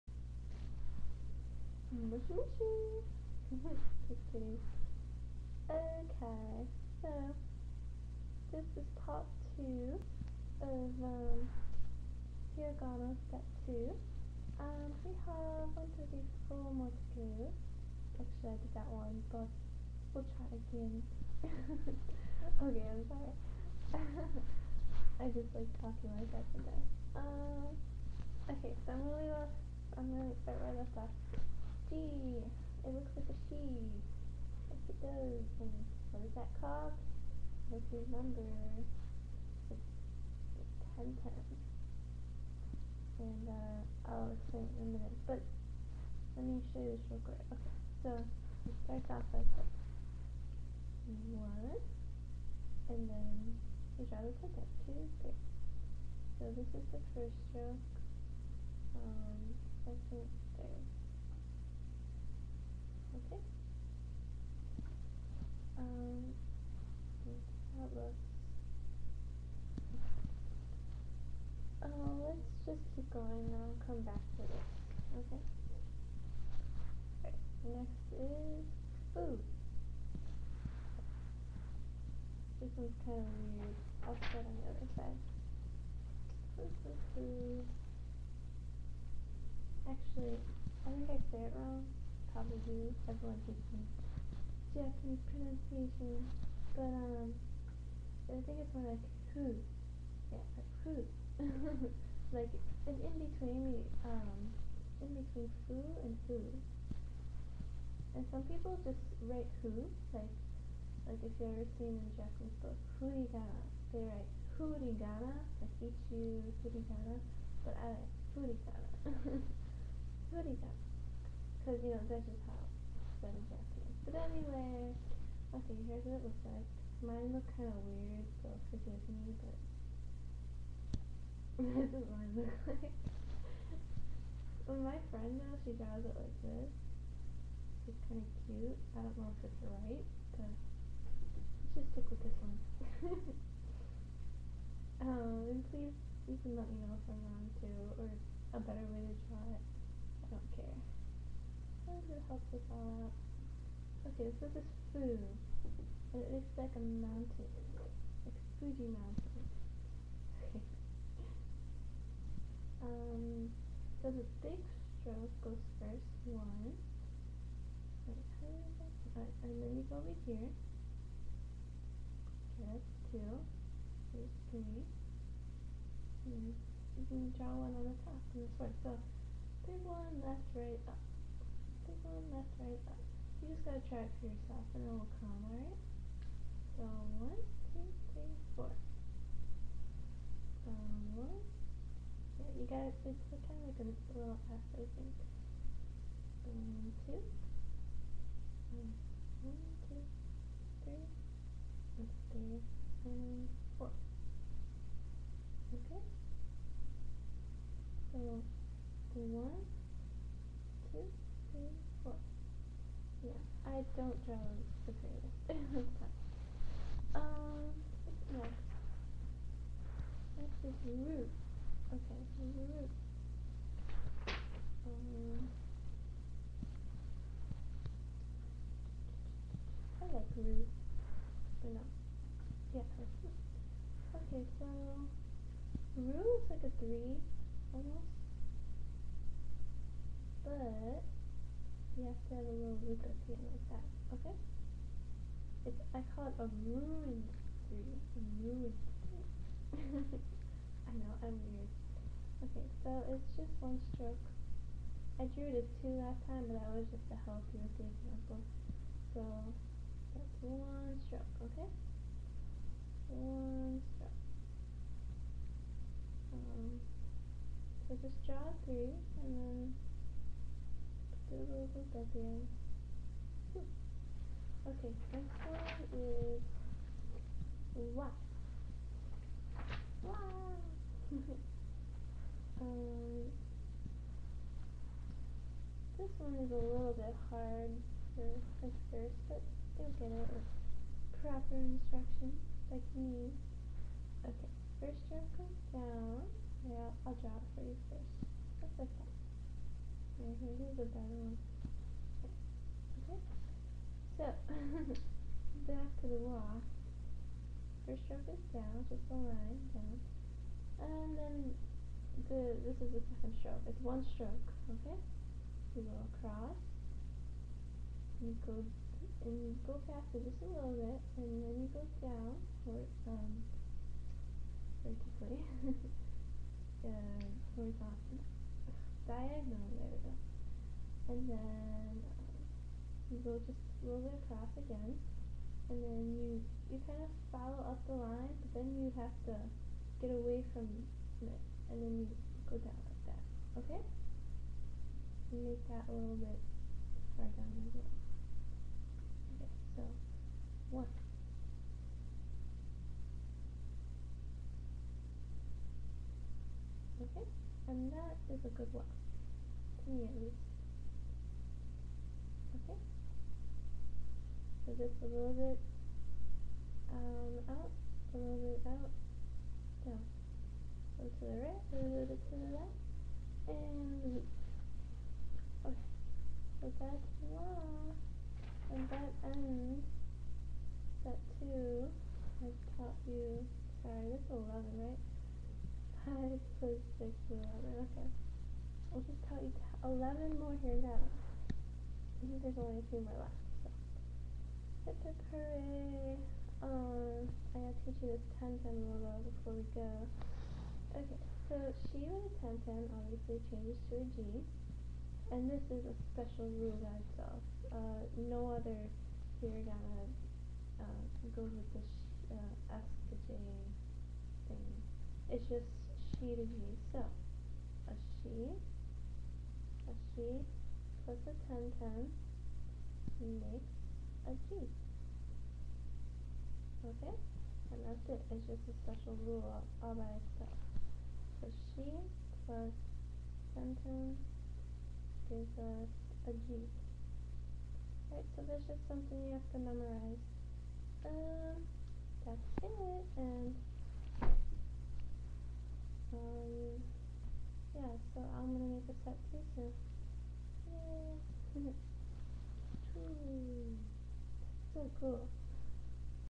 Hiragana Set 2 (Part 2)
Sorry about the sound...I'm gonna redo it all later. Well,

Other words to write: ふる(降る）to fall down (as in snow), のる（to ride, 乗る　－　as in, riding a bus or train)